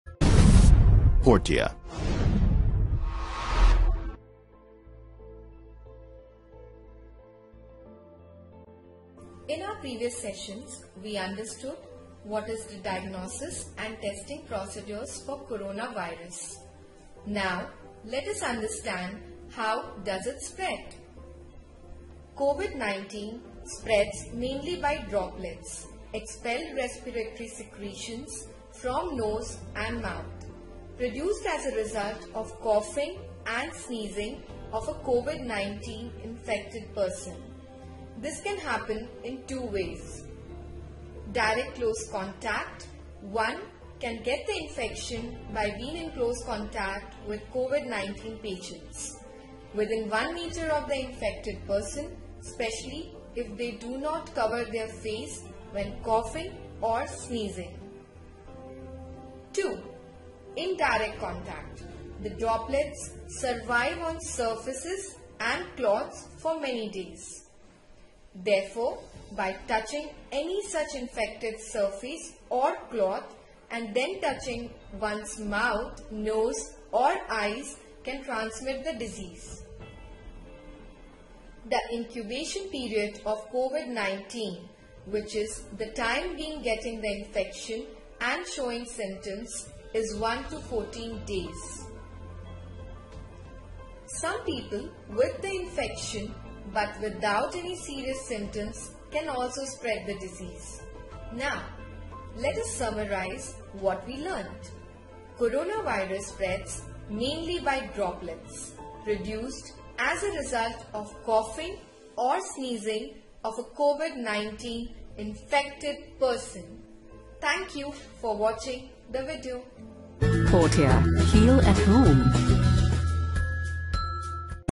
Inside The Corona Control Room by Portea Medical | How Does COVID-19 / Coronavirus Spread
As COVID-19 becomes the matter of global concern, Portea medical is taking proactive measures to help minimise the chances of spread of the coronavirus. As the safety of our patients and our healthcare workers have always been a matter of top priority for us. In this episode of 'Corona Control Room' Ritika Thirani, a healthcare worker from Portea Medical talks about 'How Does COVID-19 / Coronavirus Spread.' Click on the link below to check out other videos from the Corona Control Room Playlist